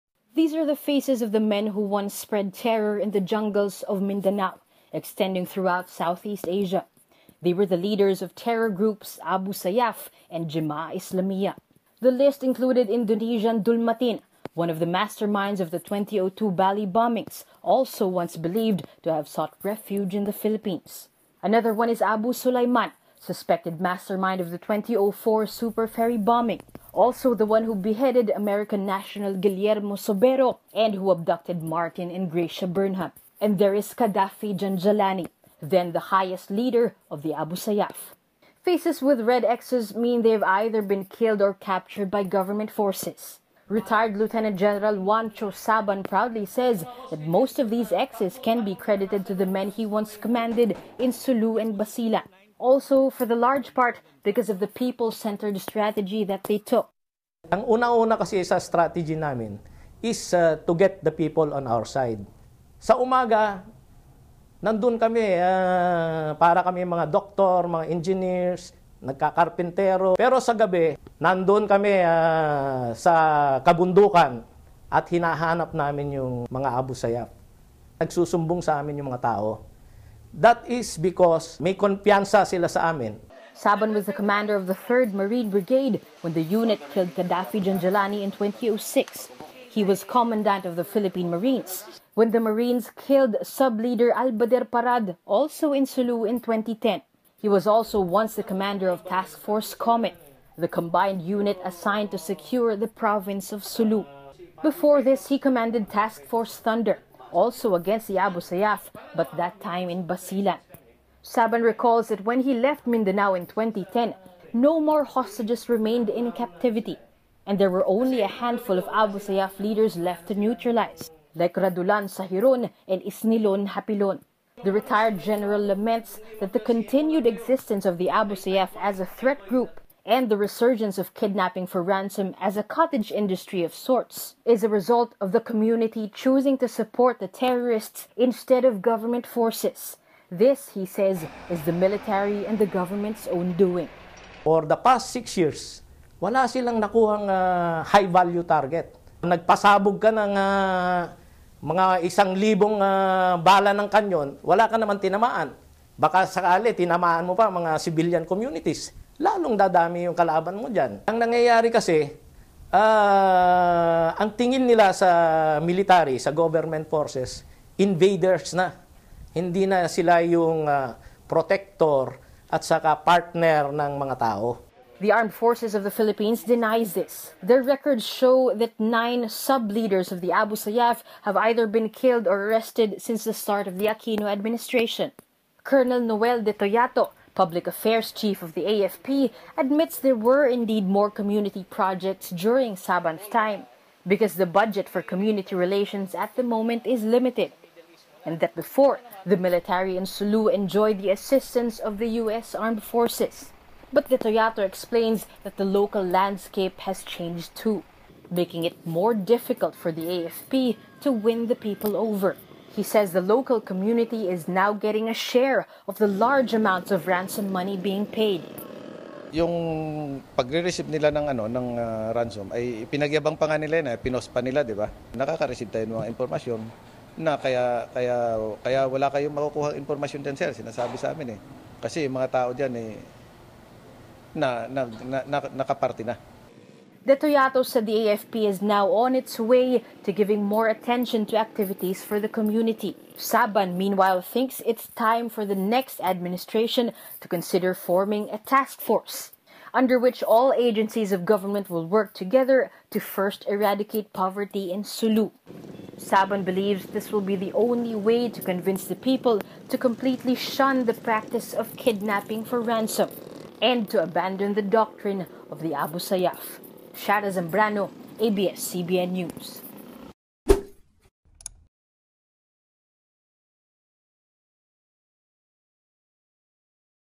Former marine general blames gov't over Abu Sayyaf resurgence
Eradicating Abu Sayyaf: Is it all about the money?